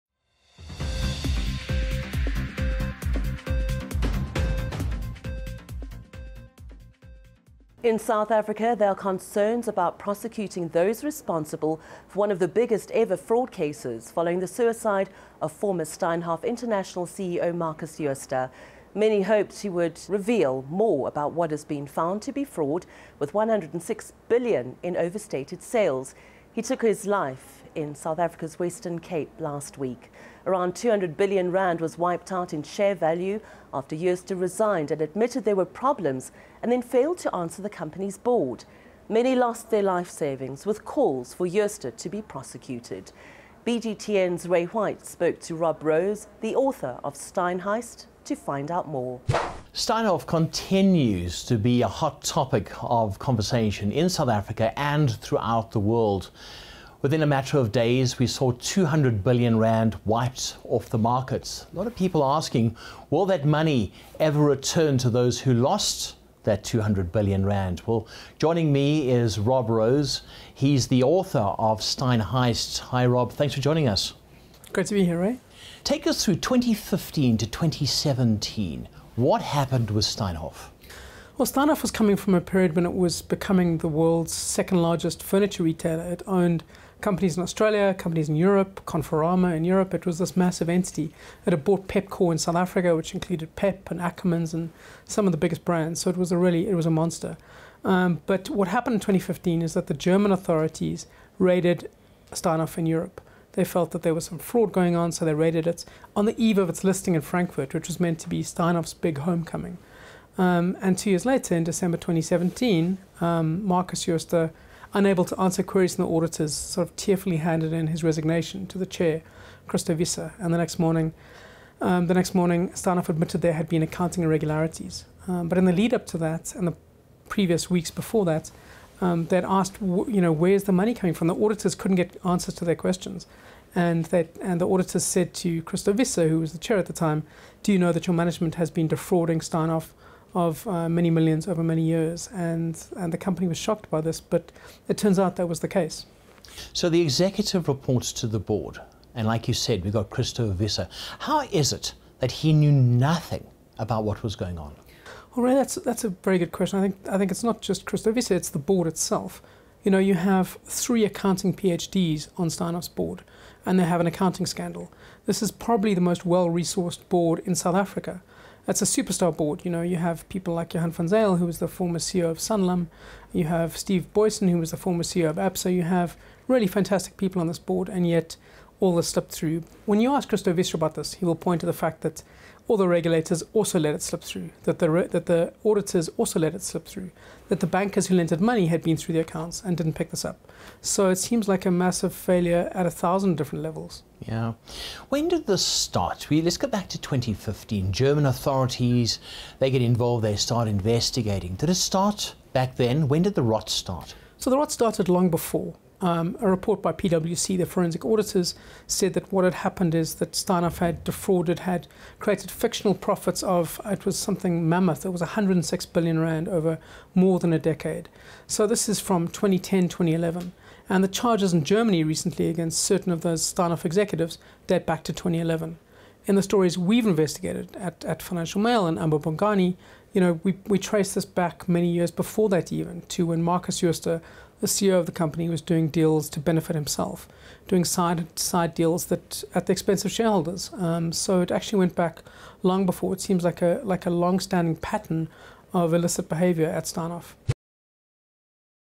Steinhoff was a monster company – and its fall was heard around the world
Rob Rose, author of Steinheist, a book and television documentary on the global scandal, spoke to BGTN in an exclusive interview following the suicide of the former Steinhoff CEO. Rose says “Steinhoff was the world’s second largest furniture retailer and owned companies in Europe, South Africa and Australia. It was a monster”.